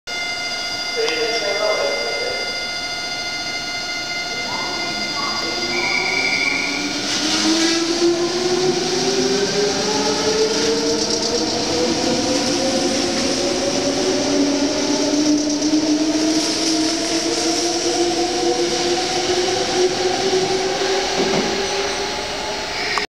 名鉄一宮駅普通須ケ口行き出発
名鉄一宮駅にて撮影。普通須ケ口行きが出発します